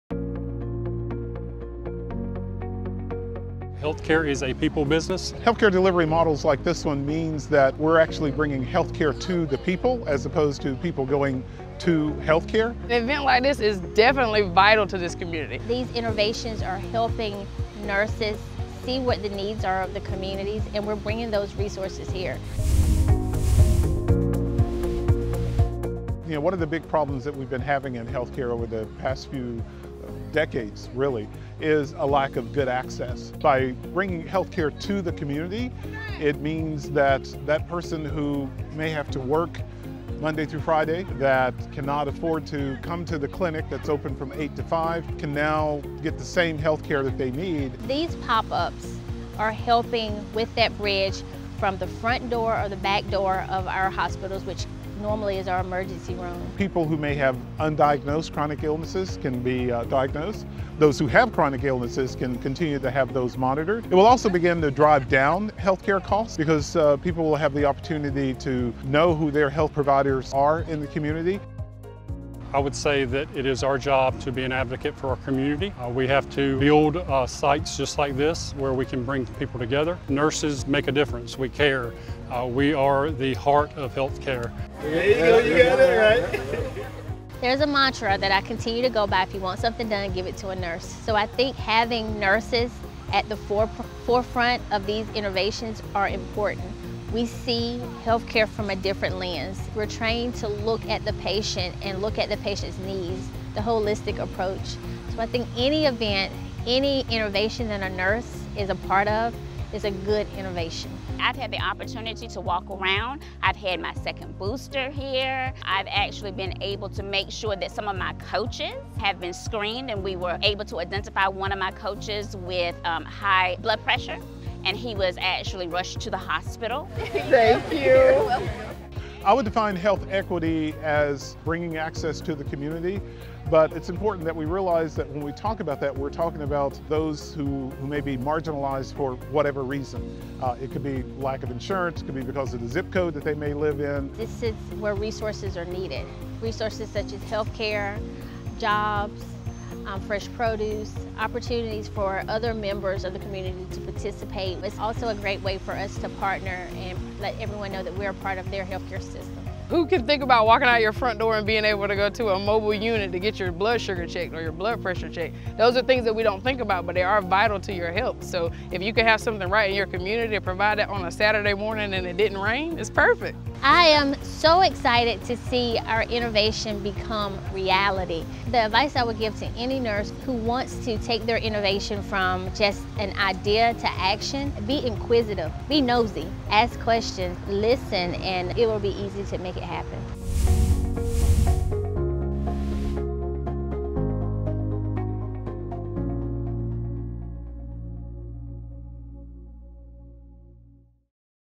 Nurse Innovation: Community Pop-Up Clinics
ANA Innovation Award (sponsored by Stryker) 2022 Individual Award Winner, KaSheta Jackson, DNP, MSN, RN, is improving health equity with her nurse-led innovation and model to improve health care delivery with CommUNITY health care pop-ups in Eastern North Carolina. These pop-ups bring health care directly to people and provide health screenings, health coaching, fresh produce, vaccines, community and health system resources, and even jobs! ANA President Dr. Ernest Grant visits a pop-up to support this nurse innovator in North Carolina.